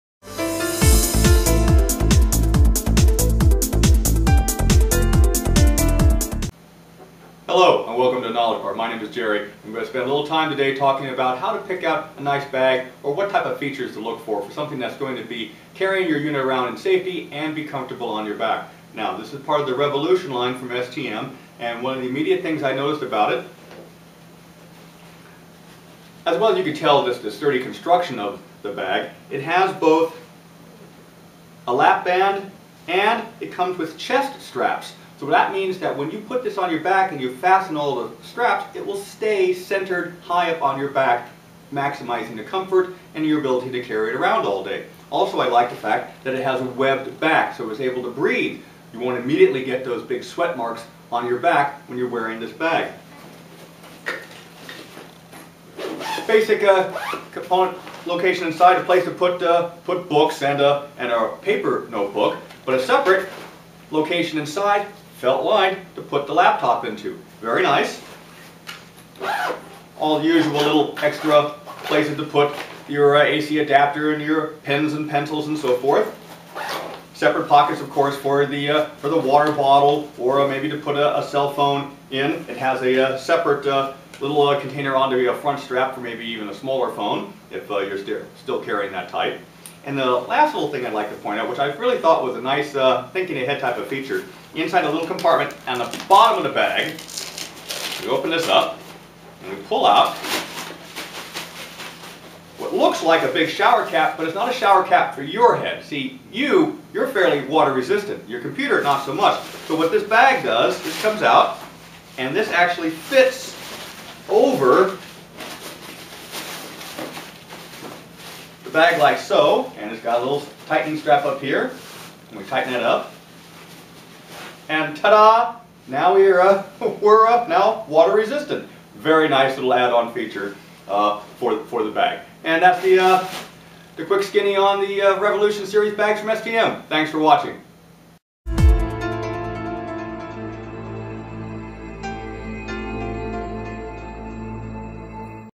Product: Review of STM Revolution Laptop Bag
Micro Center Tech Support gives you a review of the STM Revolution Laptop Bag. Which bag is right for you?